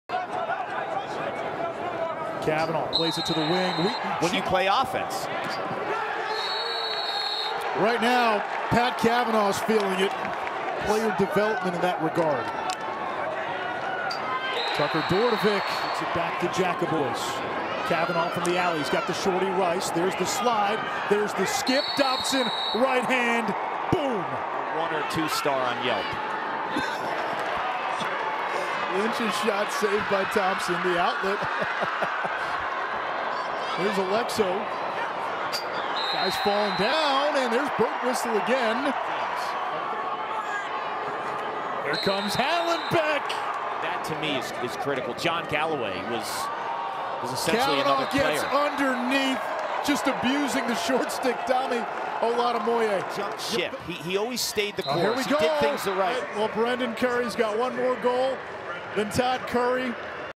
2022 NCAA HIGHLIGHTS: Notre Dame 18, Syracuse 11
Pat Kavanagh put up 10 points on his return to the Dome exactly a year after notching 10 points.